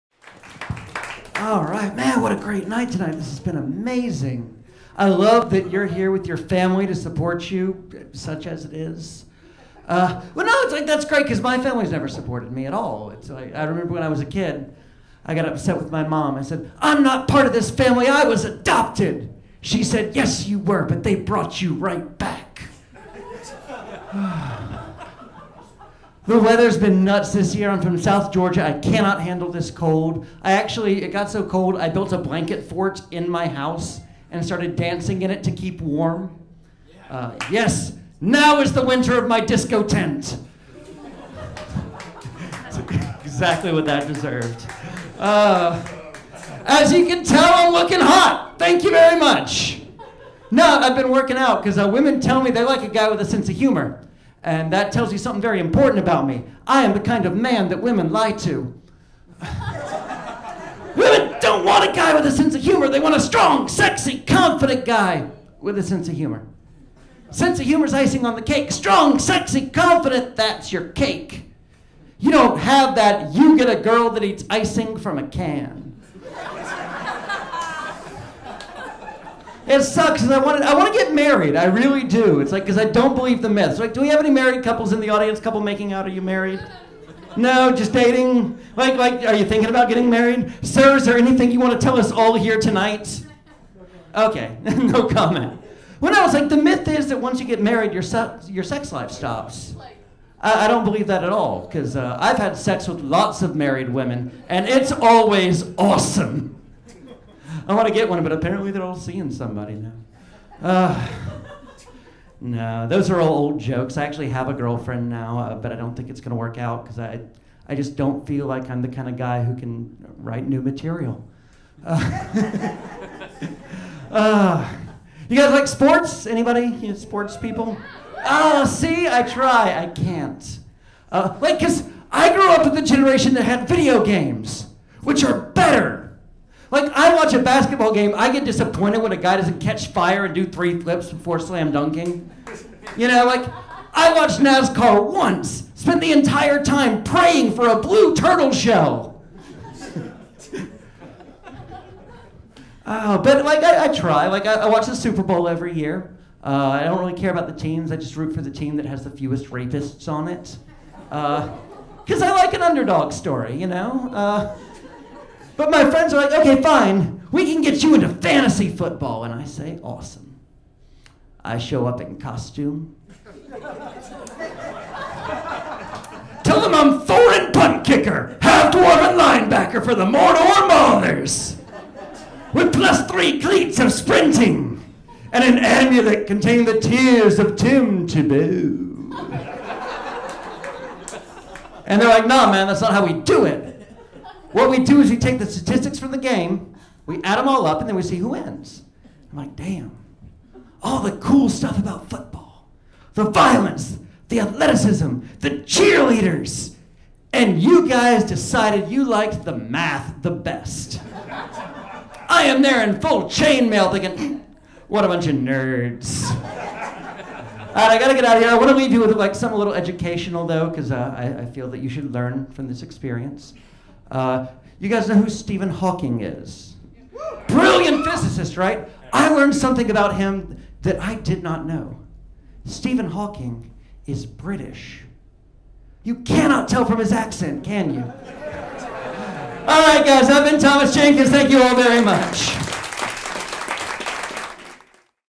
Thomas Jenkins
A short, clean set by Atlanta based comedian Thomas jenkins